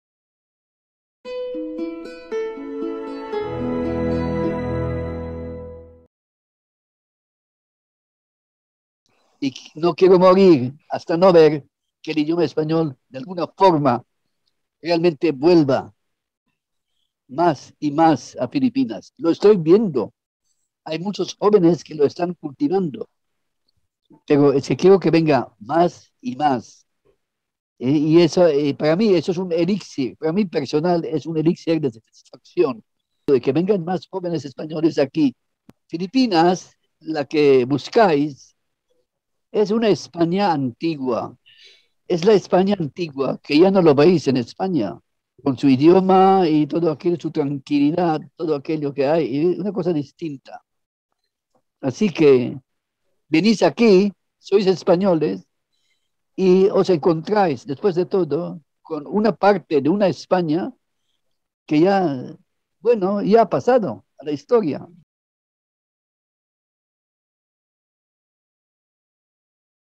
Guillermo Gómez Rivera: "Filipinas es la España antigua que ya no encontráis en España"
GUILLERMO GÓMEZ RIVERA, historiador, periodista y escritor.
Director de la Academia Filipina de la Lengua Española.

Extracto de la conferencia "La Hispanidad en Filipinas, situación actual y perspectivas futuras":